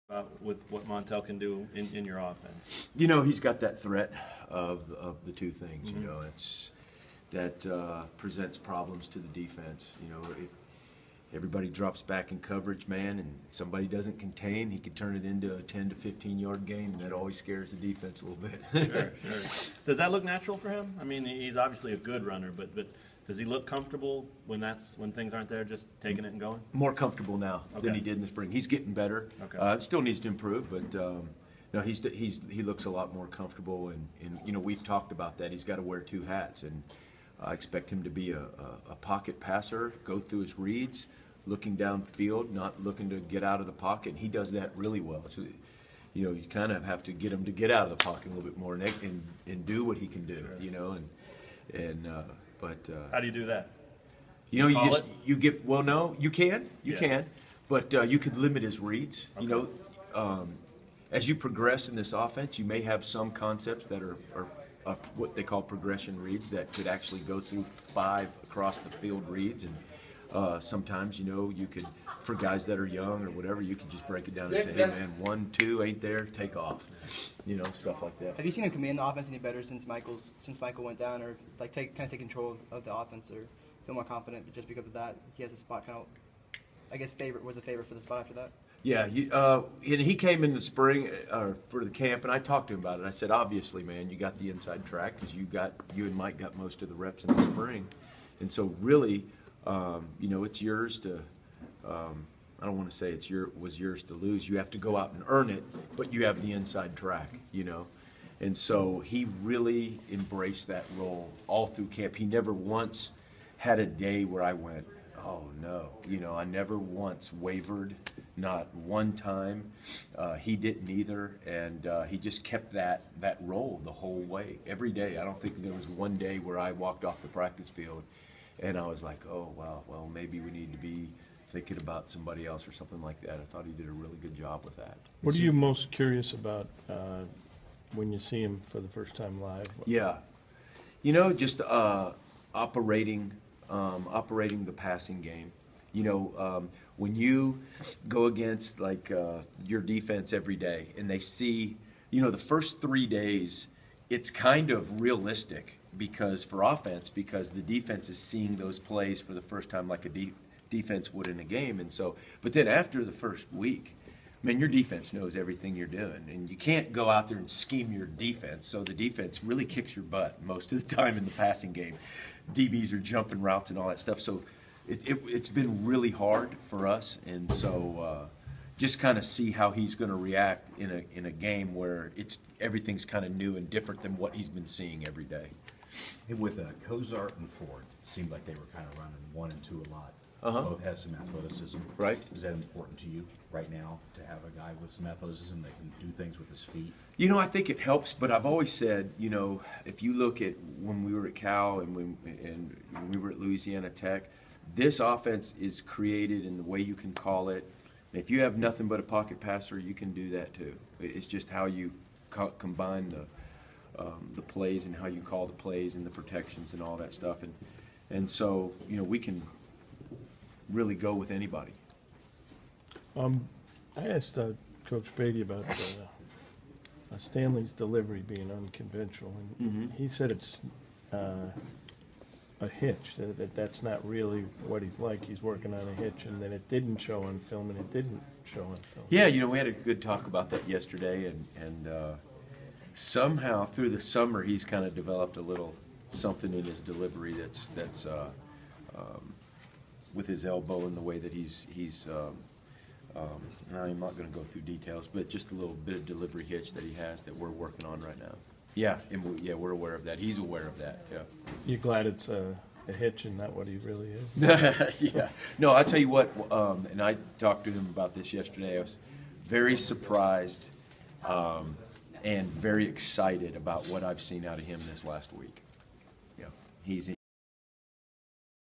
KU offensive coordinator Rob Likens on QB decision
On Monday afternoon, KU coach David Beaty announced that junior Montell Cozart would be Kansas' starting QB in the season opener. Offensive coordinator Rob Likens discussed the decision.